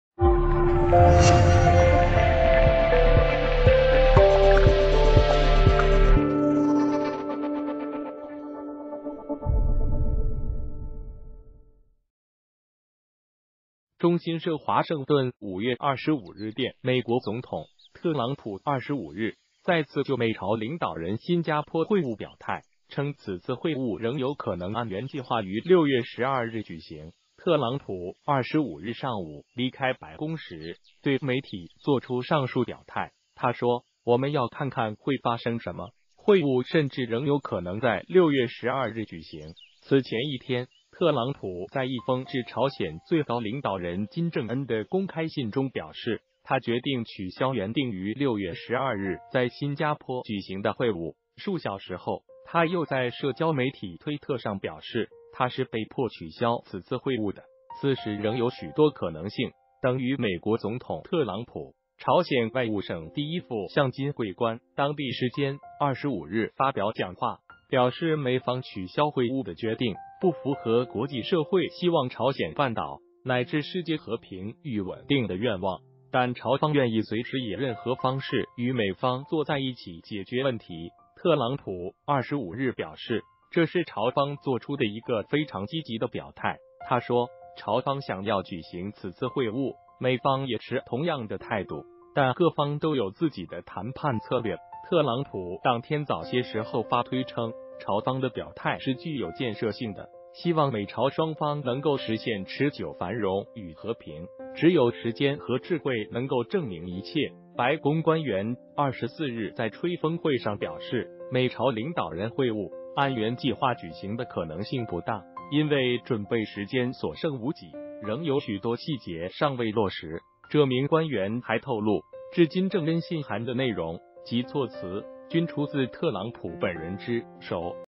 特朗普称美朝领导人会晤仍可能按原计划举行
谢谢！中新社华盛顿5月25日电美国总统特朗普25日再次就美朝领导人新加坡会晤表态，称此次会晤仍有可能按原计划于6月12日举行。特朗普25日上午离开白宫时对媒体作出上述表态。他说，“我们要看看会发生什么，会晤甚至仍有可能在(6月)12日举行。”此前一天，特朗普在一封致朝鲜最高领导人金正恩的公开信中表示，他决定取消原定于6月12日在新加坡举行的会晤。数小时后，他又在社交媒体推特上表示，他是被迫取消此次会晤的，此事仍有许多可能性。=美国总统特朗普。朝鲜外务省第一副相金桂冠当地时间25日发表讲话，表示美方取消会晤的决定不符合国际社会希望朝鲜半岛乃至世界和平与稳定的愿望，但朝方愿意随时以任何方式与美方坐在一起解决问题。特朗普25日表示，这是朝方作出的一个非常积极的表态。他说，朝方想要举行此次会晤，美方也持同样的态度，但各方都有自己的谈判策略。特朗普当天早些时候发推称，朝方的表态是具有建设性的。希望美朝双方能够实现持久繁荣与和平。只有时间和智慧能够证明一切。白宫官员24日在吹风会上表示，美朝领导人会晤按原计划举行的可能性不大，因为准备时间所剩无几，仍有许多细节尚未落实。这名官员还透露，致金正恩信函的内容及措辞均出自特朗普本人之手。(完)